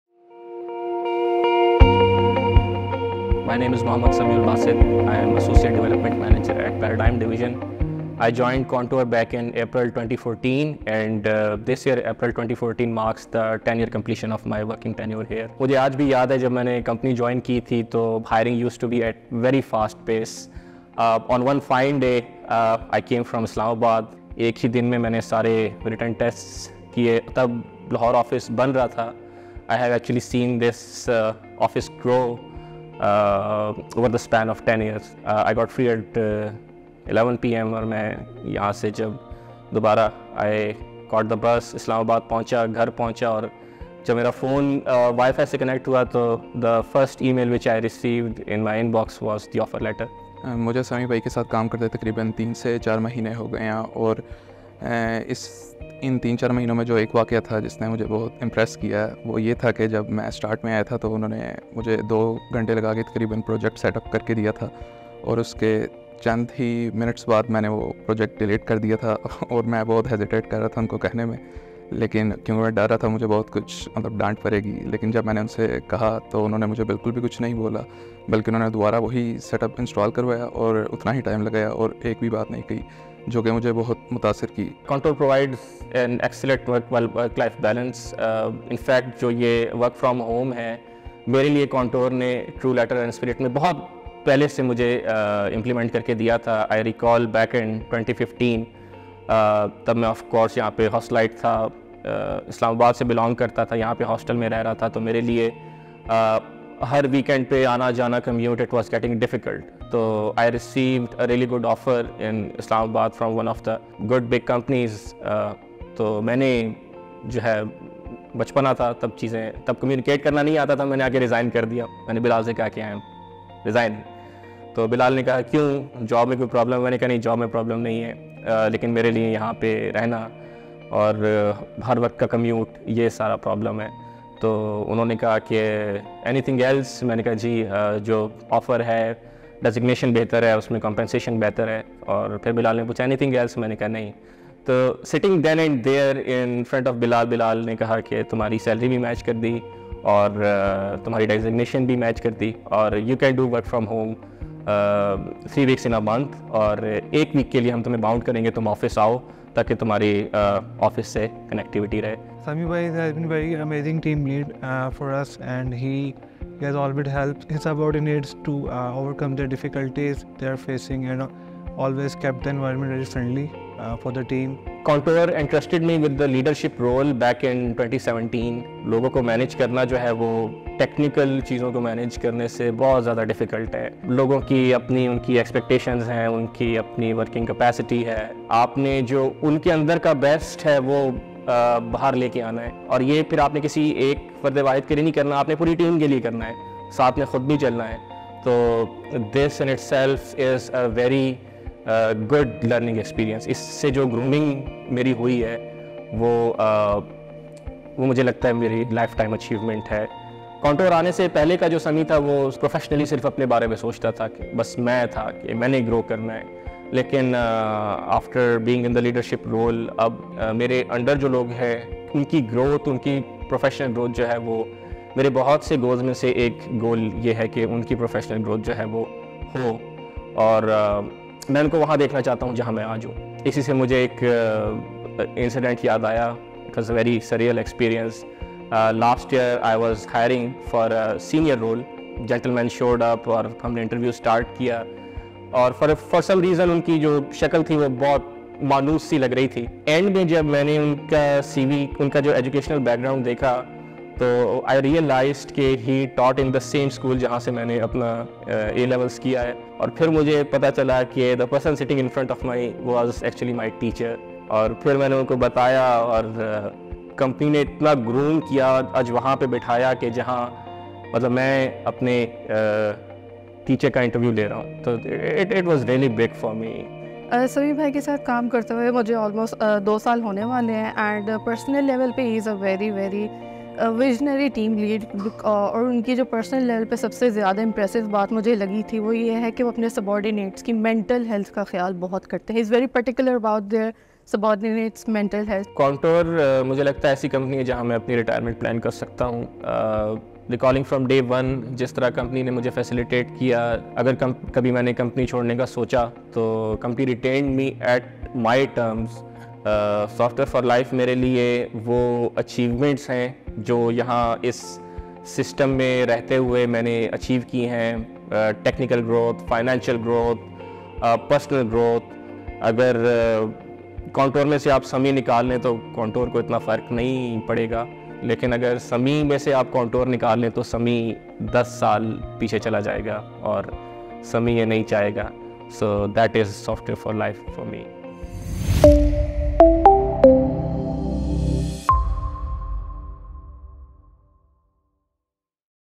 Celebrating 10 Years of Tenure | Sami Basit
Celebrating a Decade of Dedication! 🙌
In continuation of our Annual 10-Year Tenure Celebrations,🥳 join us in honoring Sami Basit, our Associate Development Manager, as he completes an incredible 10-year milestone with us! 👏 🙌
Here's to many more years of success and growth!